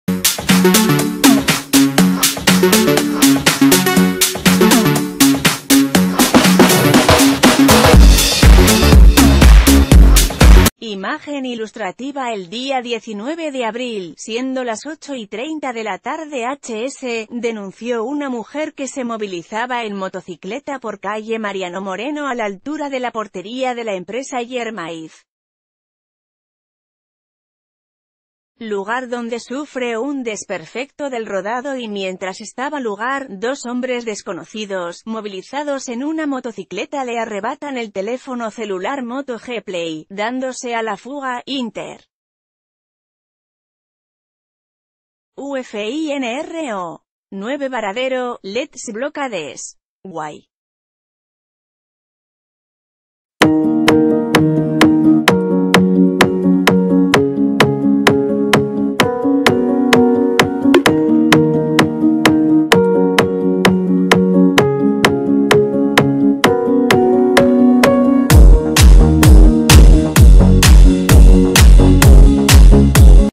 Policiales: Se le rompió la moto y le robaron el celular
Imagen Ilustrativa El día 19 de Abril, siendo las 20:30 hs, denunció una mujer que se movilizaba en motocicleta por calle Mariano Moreno a la altura de la portería de la empresa Germaiz, lugar donde sufre un desperfecto del rodado y mientras estaba lugar, dos hombres desconocidos, movilizados en una...